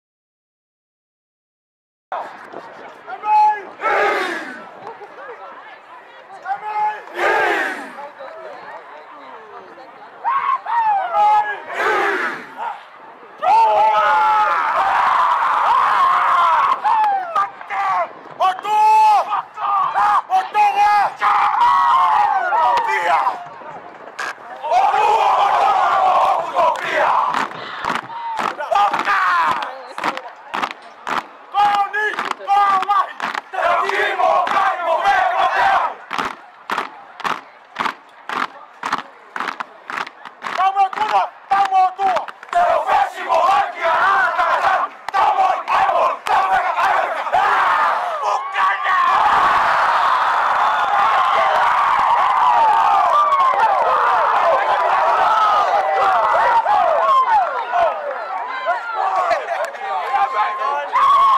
Haka - Menlo-Atherton Varsity Football 2018-08-31
LOS ALTOS HILLS, CA, August 31, 2018. Haka performed by the Menlo-Atherton Bears before defeating the Archbishop Mitty High School Monarchs 28-21.